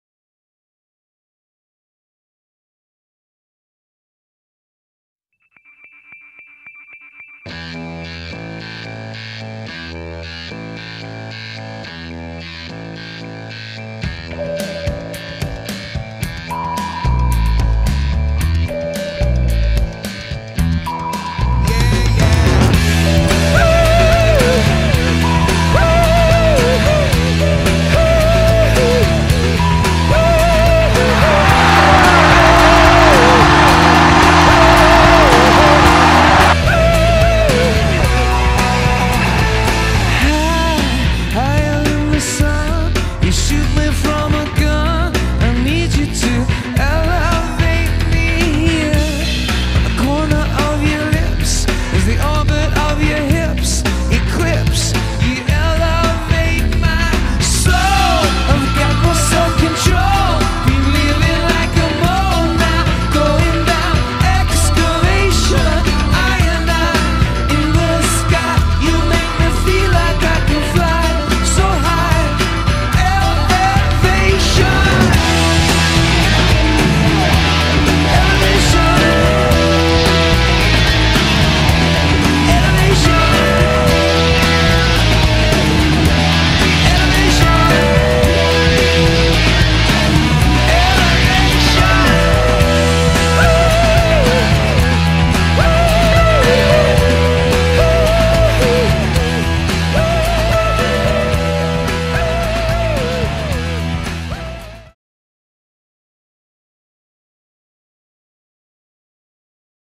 BE Hockey - Elevation (2002)
Bishop Eustace Ice Hockey
Highlights of the win over Saint Augustine.
Original Air Date: January 14, 2002